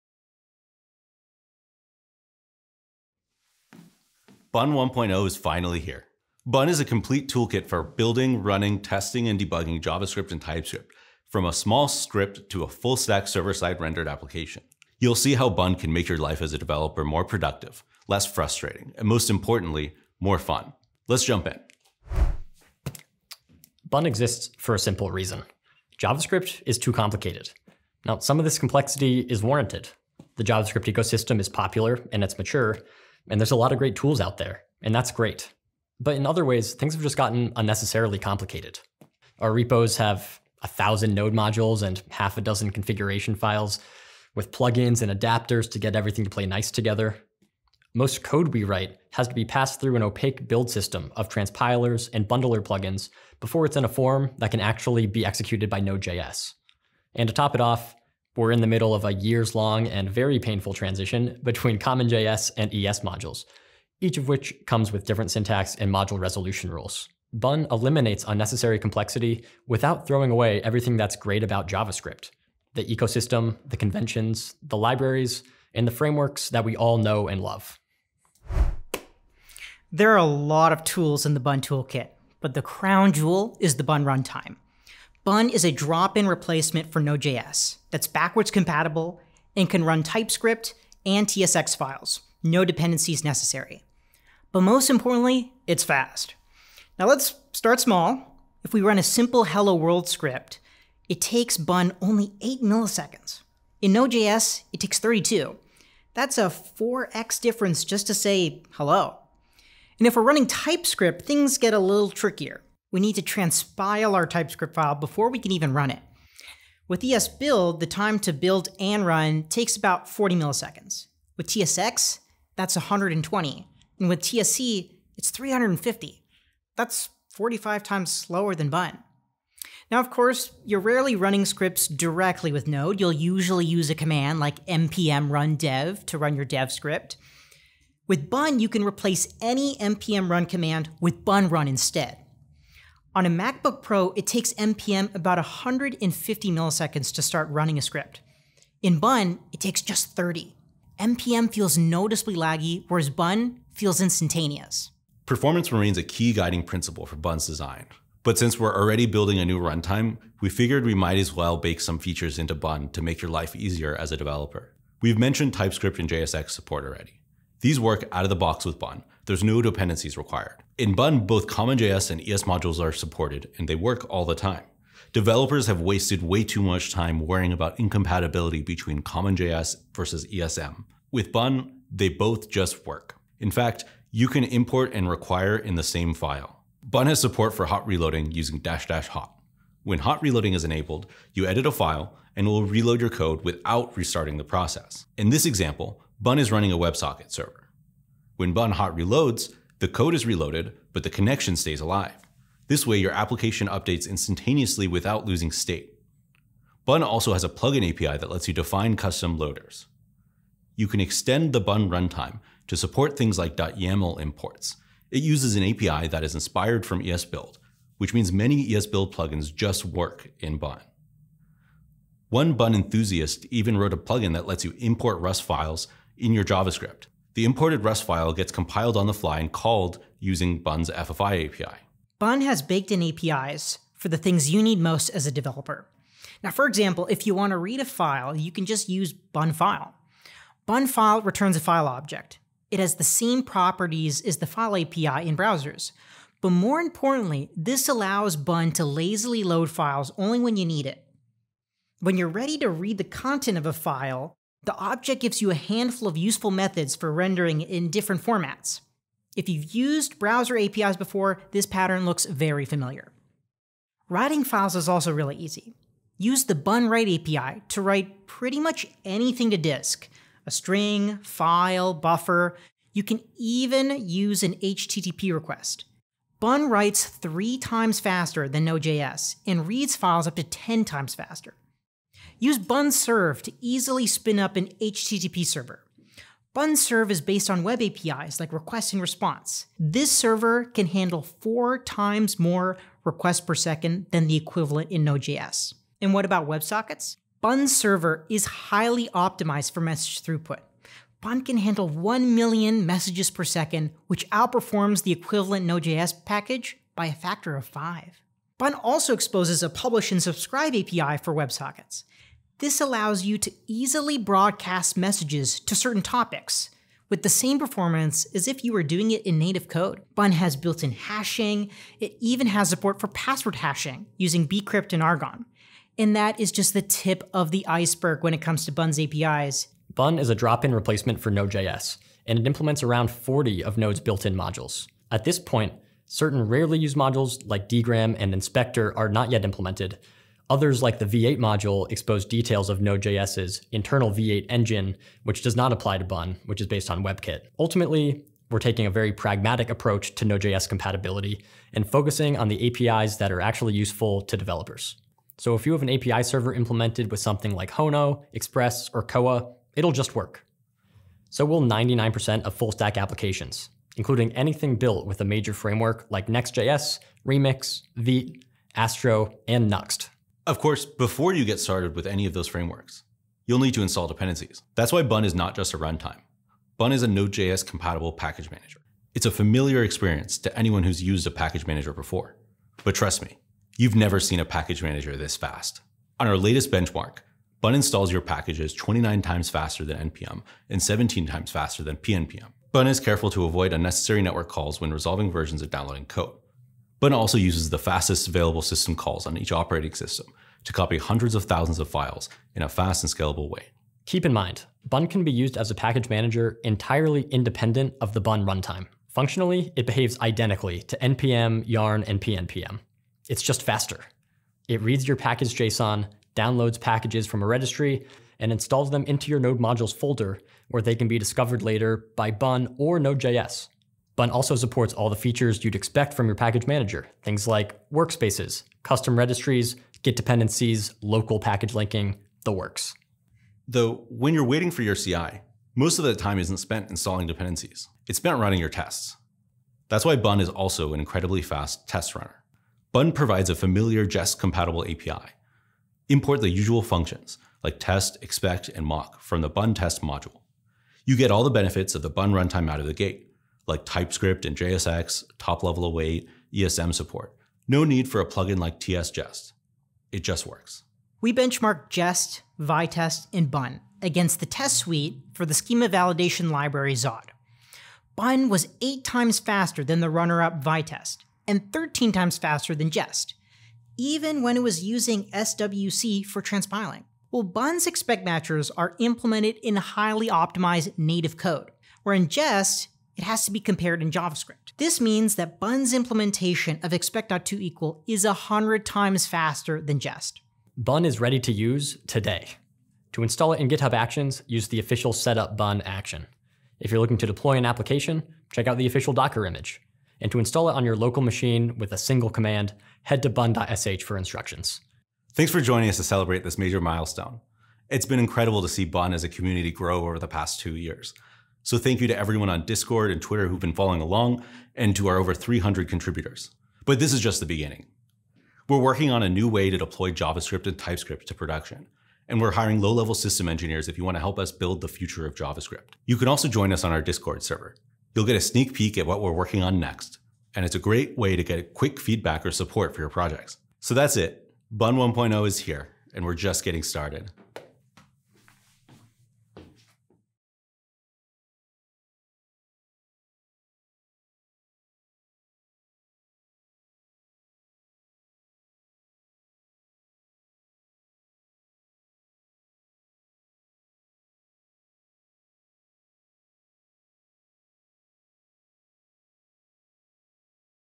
Bun 1.0 is here
Bun is an all-in-one JavaScript runtime & toolkit designed for speed, complete with a bundler, test runner, and Node.js-compatible package manager.